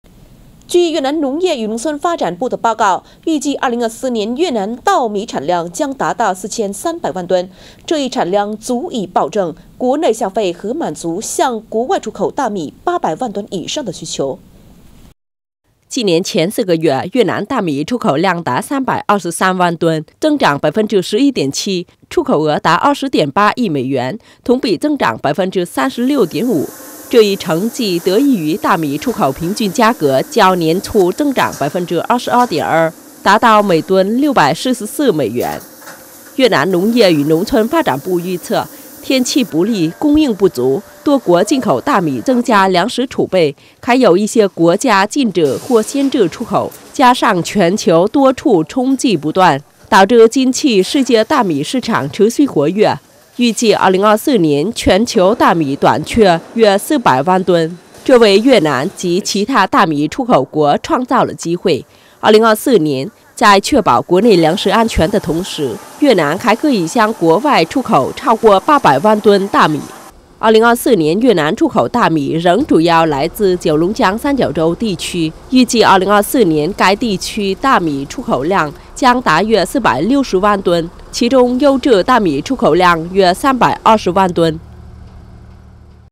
2024年越南大米出口有望超过800万吨
据越南农业与农村发展部的报告，预计2024年越南稻米产量将达到4300万吨。这一产量足以保证国内消费和满足向国外出口大米800万吨以上的需求。